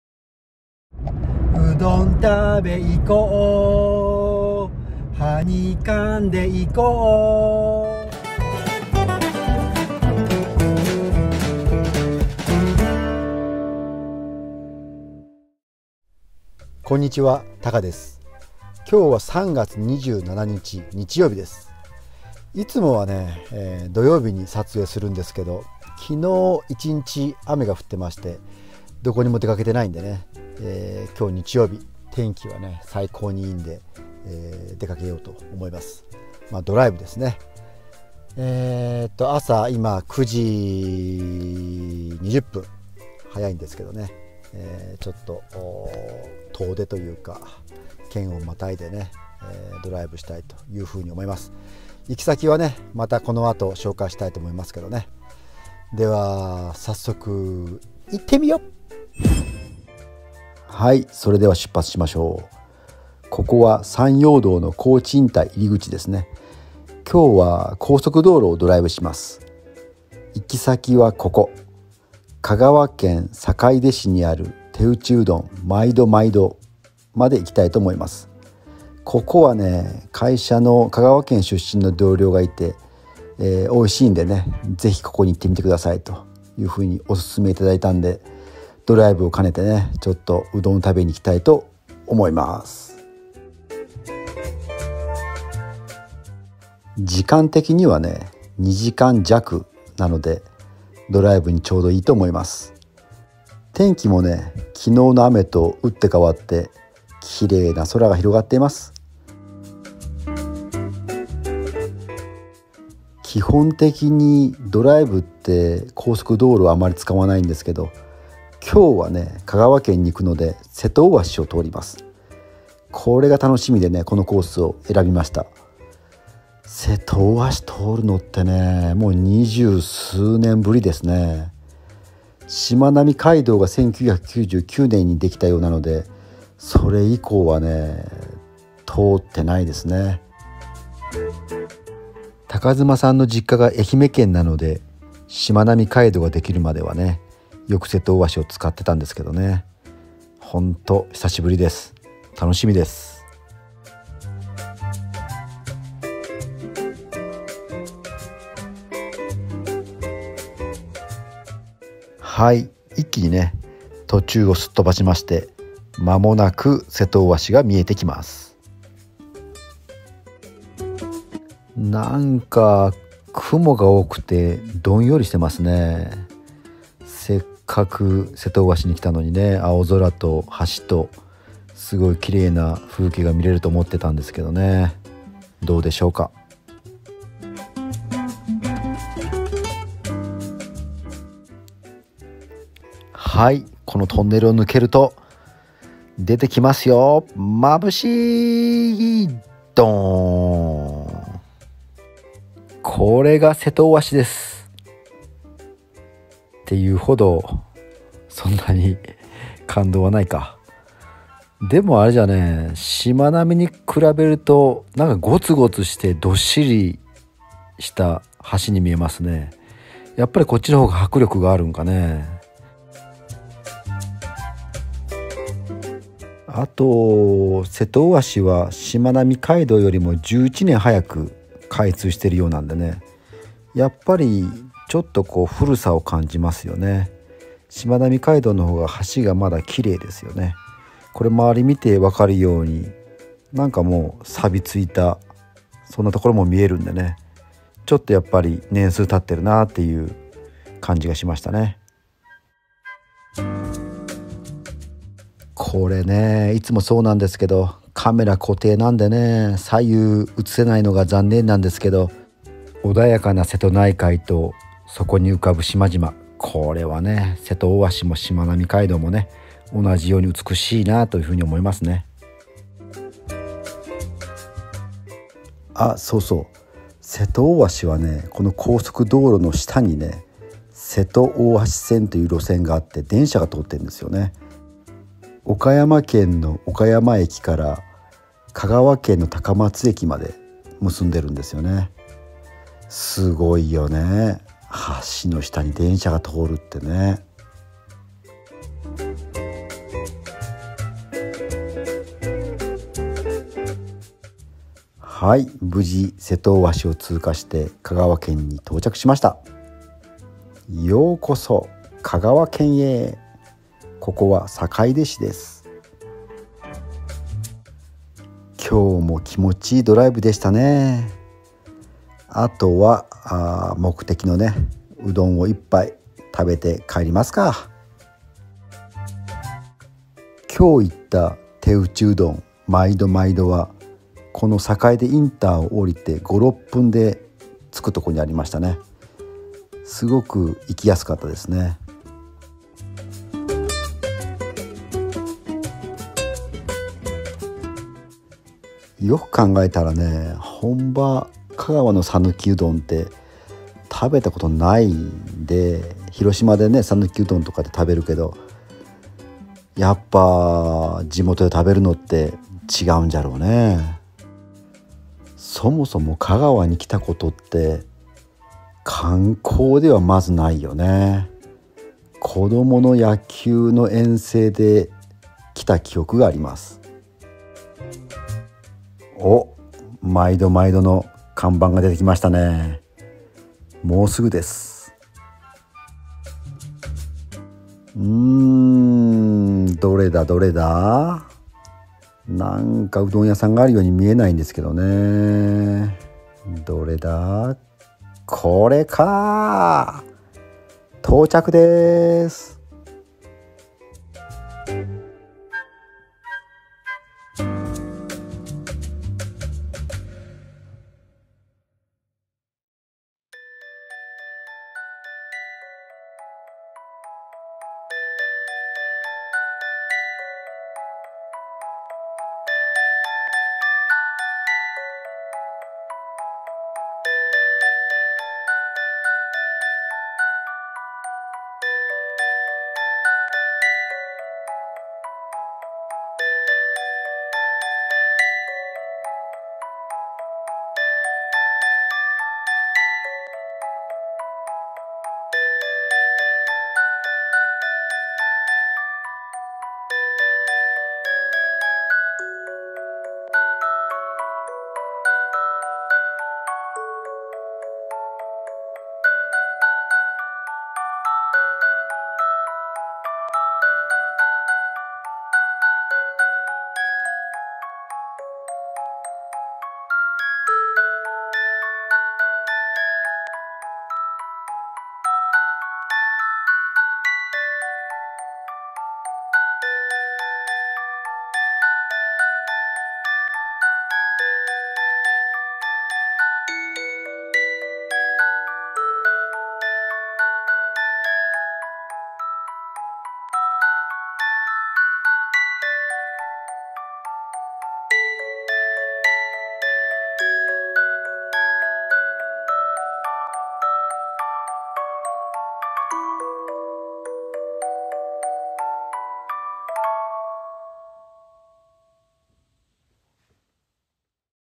#104 TAKAさん 一杯のうどんを求めて香川へ🚗
天気がいい日はやっぱりドライブをしたくなる。
少し足を延ばして香川県へ。
ここを選んだのは瀬戸大橋を通りたかったこと、そして、本場の讃岐うどんを食べたかったこと。
今日も大満足じゃ〜。